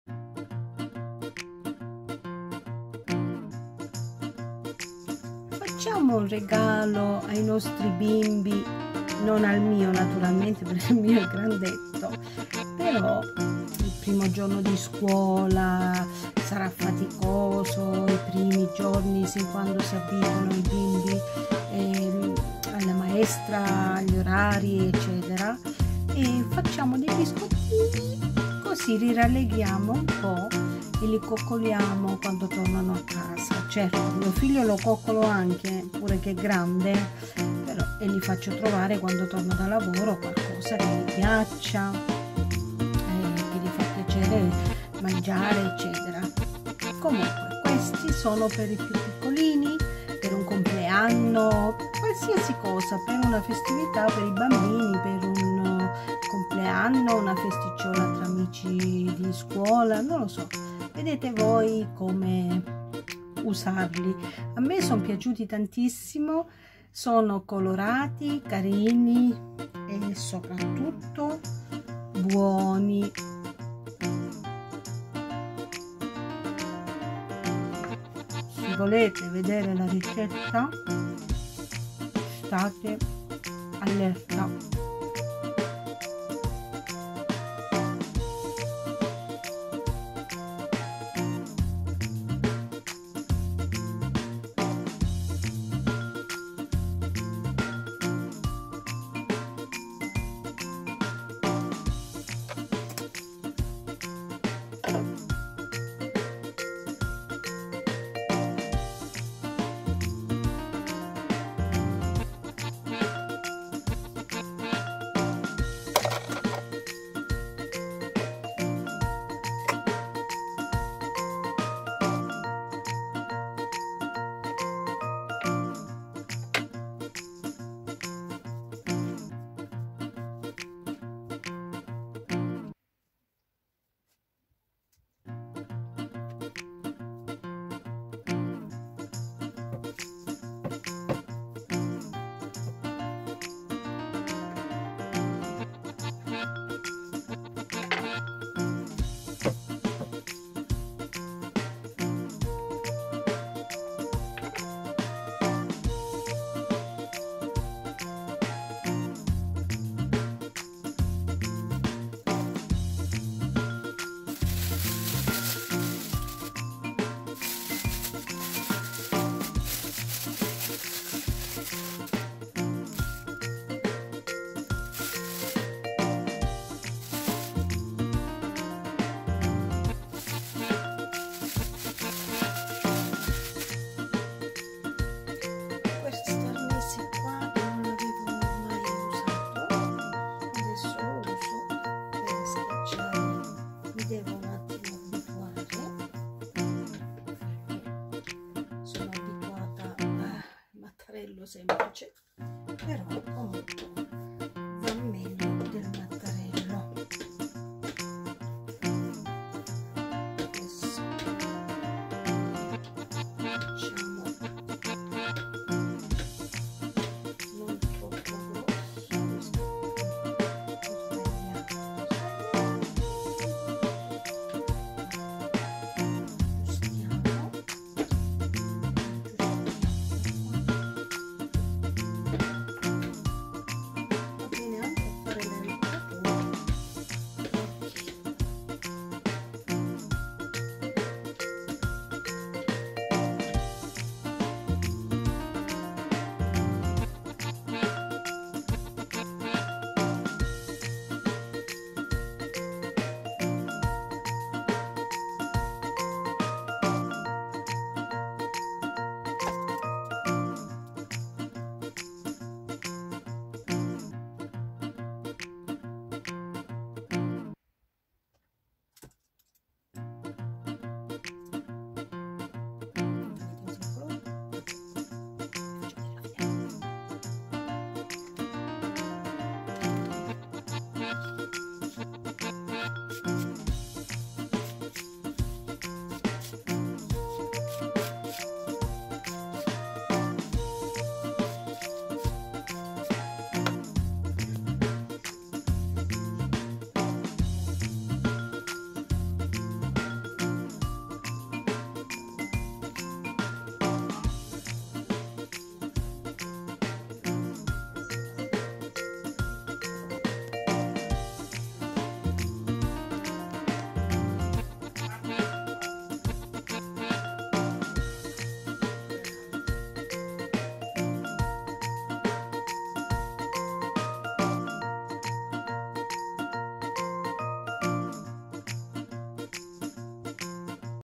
Matitoni di pasta frolla
ingredienti

100 gr di burro
250 gr farina
1 tuorlo
100 gr zucchero a velo.
1 cucchiaino aroma vaniglia
1 cucchiaio di acqua
coloranti alimentari a piacere

forno statico
200 °  20 minuti circa

volevo fare dei biscotti semplici poi ho visto che la scuola è iniziata da poco e mi sono ricordata di questi matitoni che non so quando li avevo visti su una rivista e vaiiii subito le mani in pasta 😂😂😂
a me piacciono mettono allegria e sono anche buonissimi. provare per credere

se vi piace la ricetta
👉un commento
nessuna novità
❤ grazie
👩‍🍳alla prossima
baci
Mary Gio